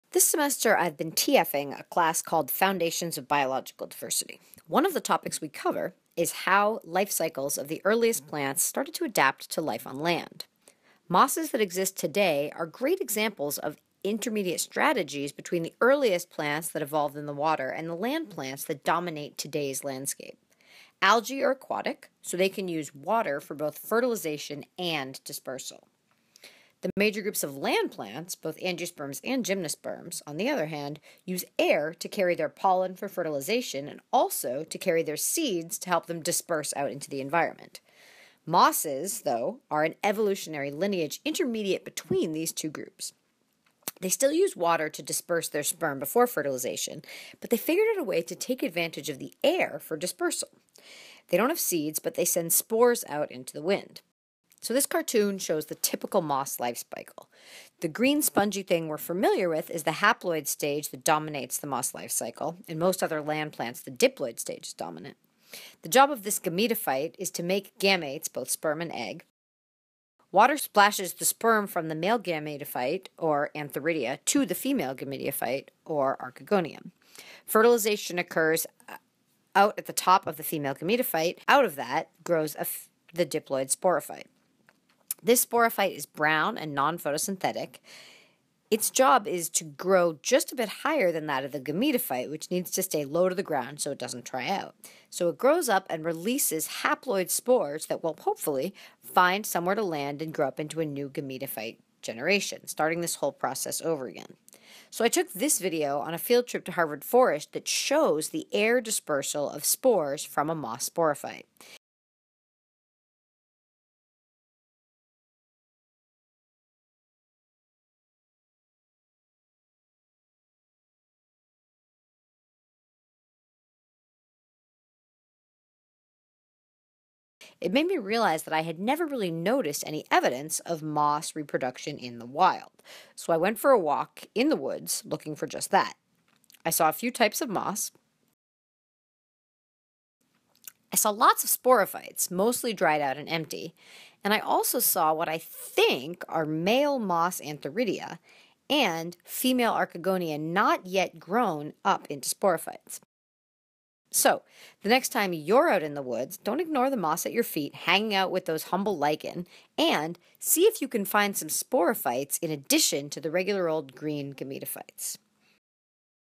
Moss Life Cycle
I made this video while a grad student at Harvard TAing an introductory biology course. It is a bit rough, as it was one of my first attempts a creating a video like this. It is an overview of moss reproduction in the context of plant evolution. All photos are my own.